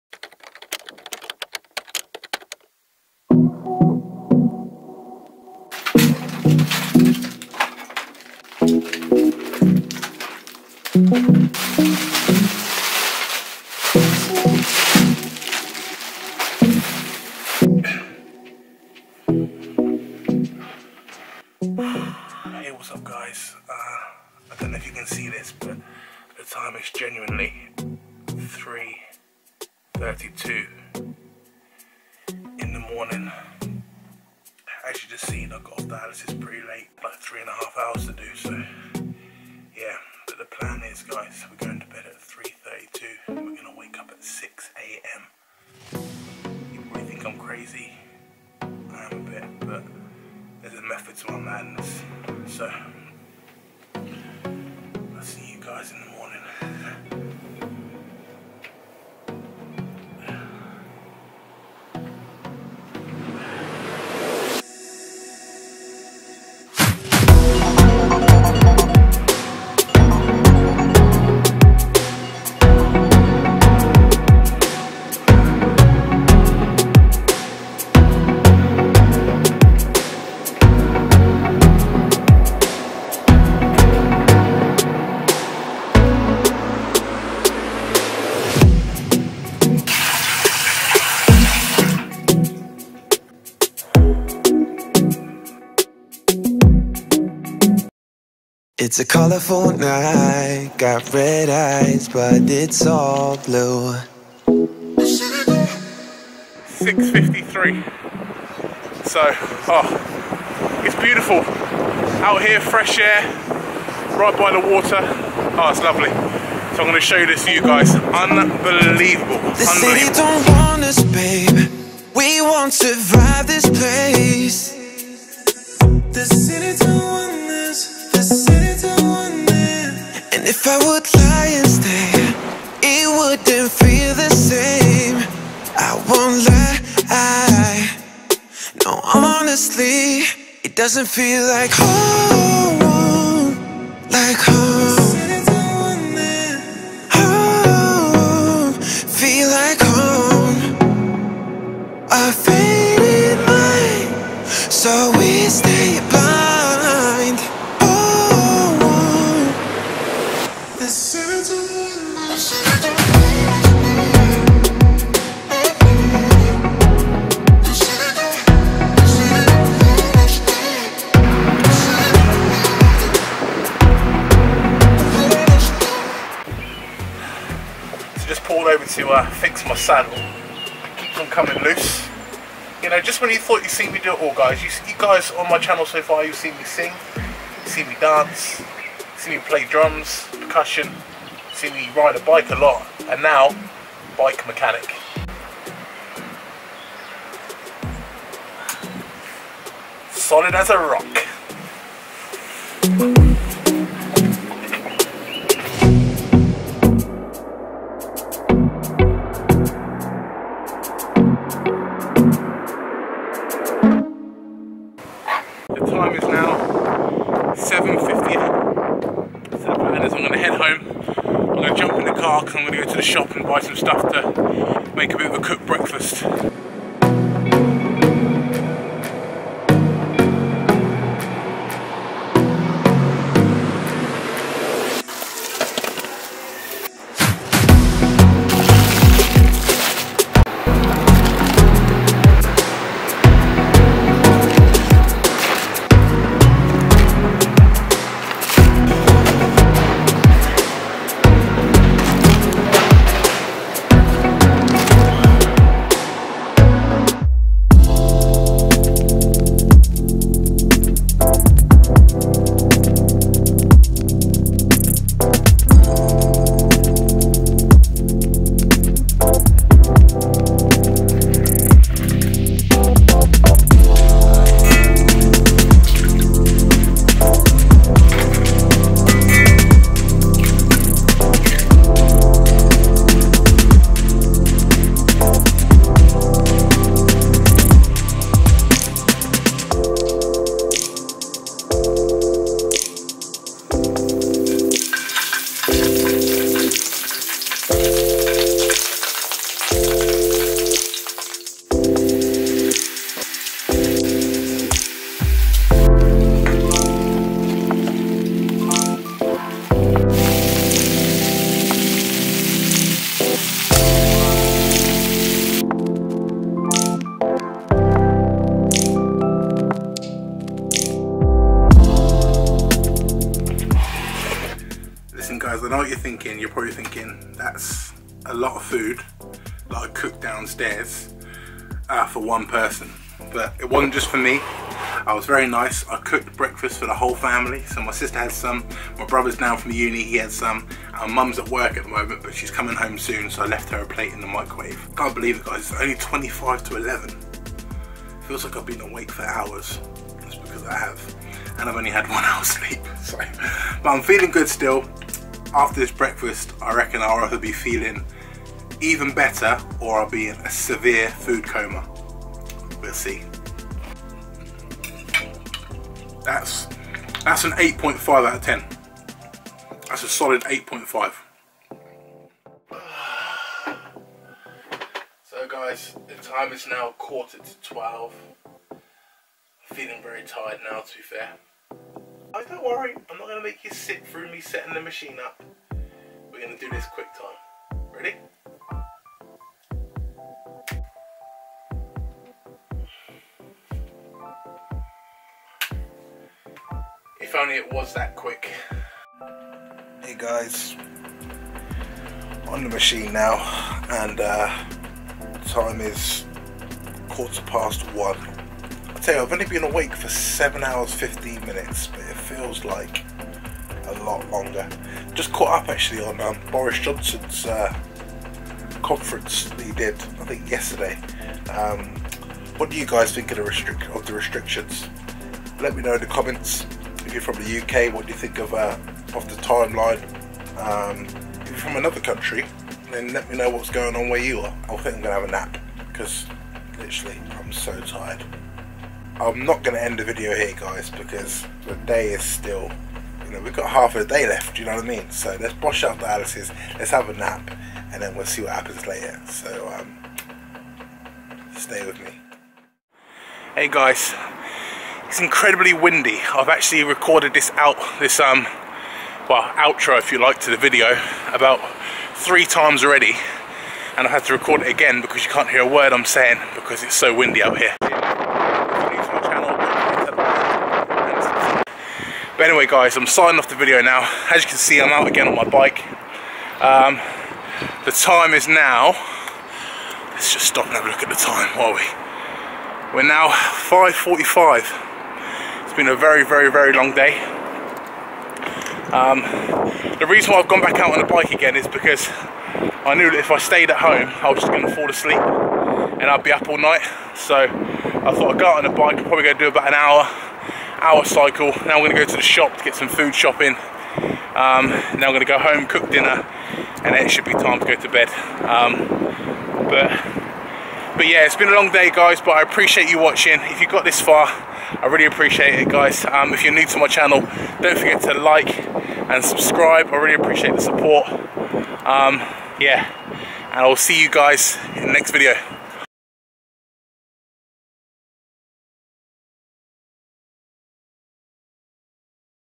Day In The Life Vlog |Up And Out At 6am
Thought I'd see how productive a day I could have on just over an hours sleep... Here's what happened?